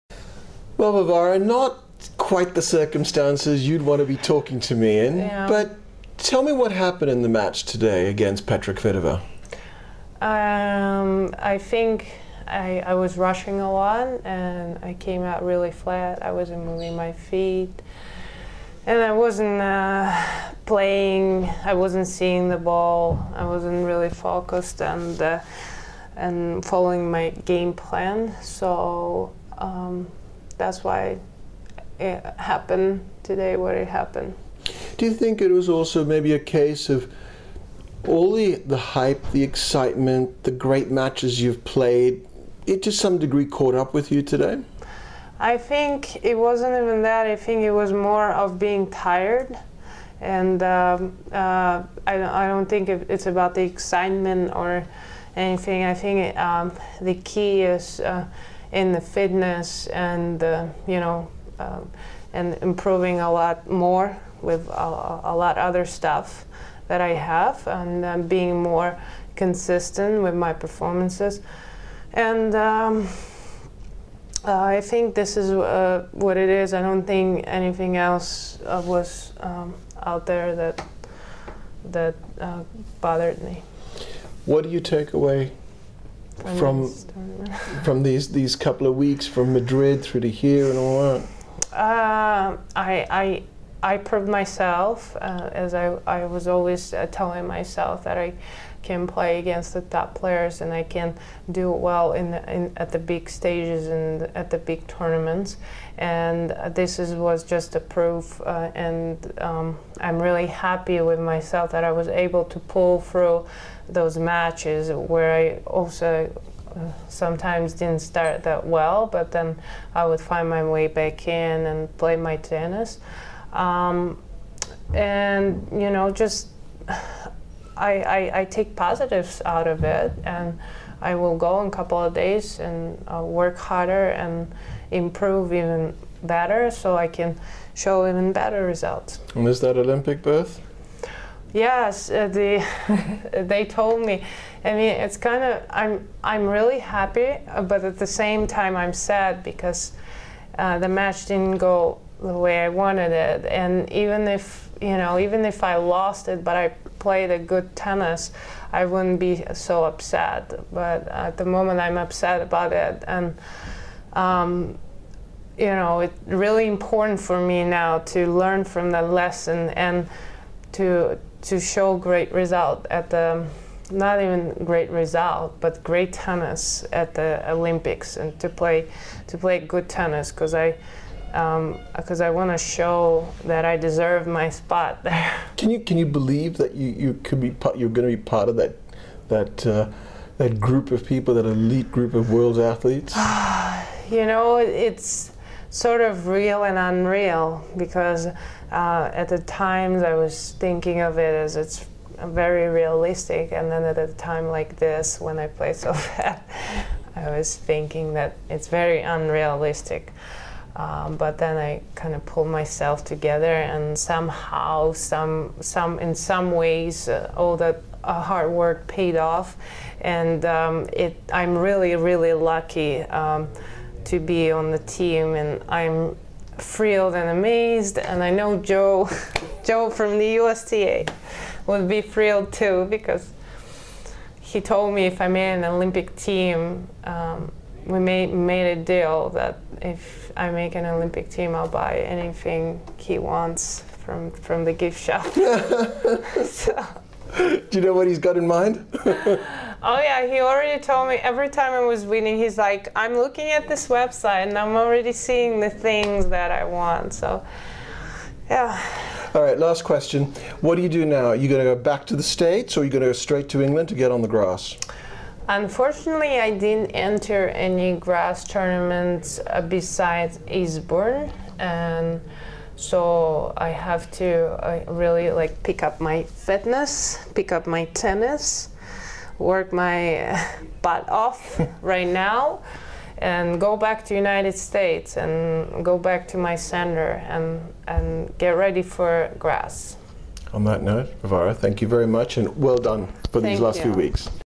2012 French Open: Varvara Lepchenko, Fourth Round
Craig Gabriel catches up with the final American player in either singles draw, Varvara Lepchenko, about her defeat at the hand of 2011 Wimbledon champion & Roland Garros fourth seed Petra Kvitova in straight sets. The 26-year-old reflects on her progress in the tournament and where she expects to go from the best showing of her career at a Grand Slam.

For exclusive recaps, videos and more from Roland Garros, check out